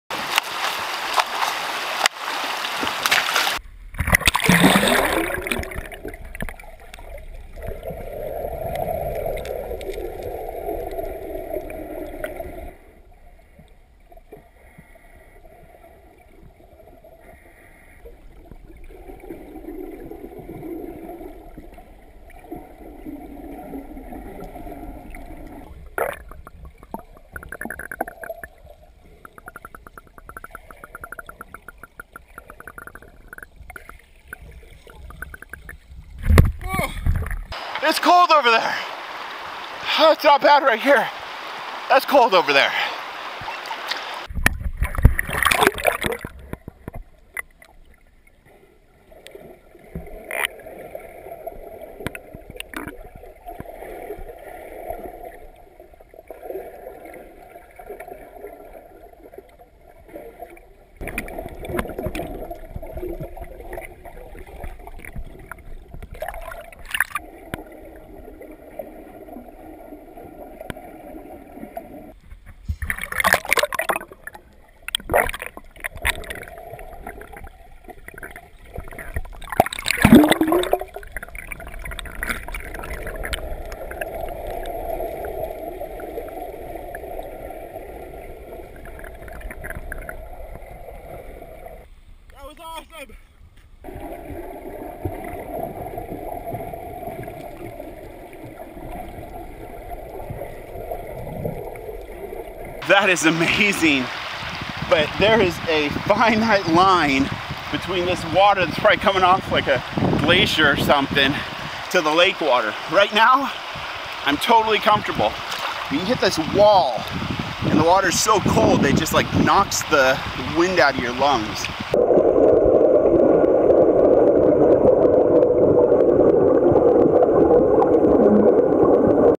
Best of Alaska ~ Snorkeling with Salmon!
Snorkeling with sockeye salmon in Seward, Alaska.